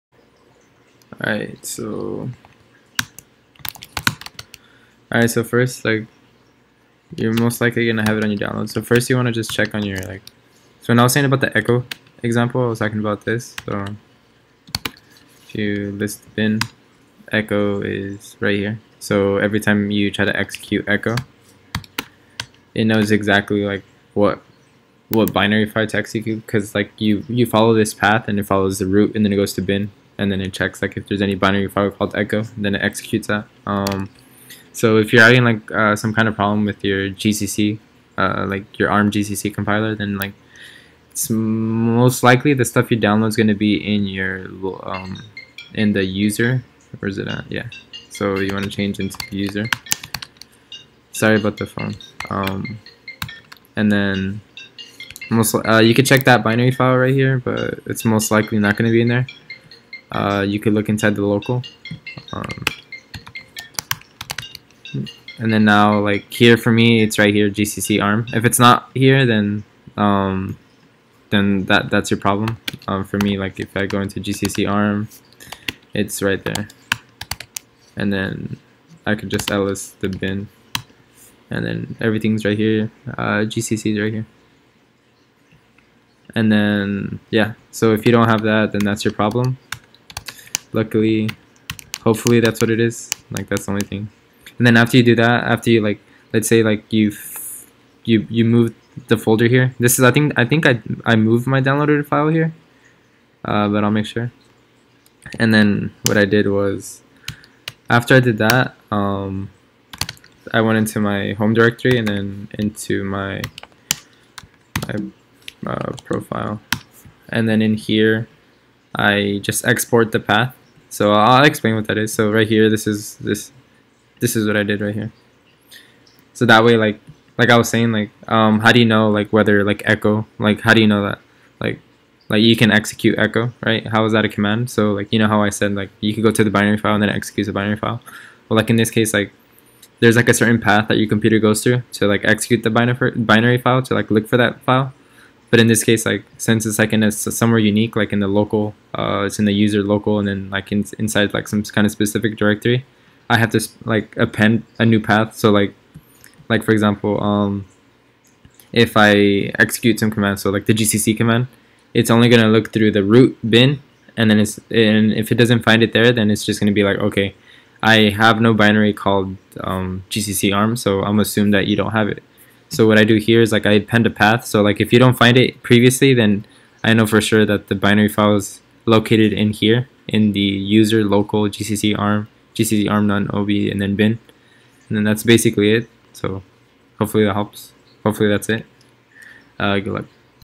gcc-arm-none-eabi command not found (1/2)
There are more paths that the computer goes through but for simplicity I just assumed the only path is "\bin"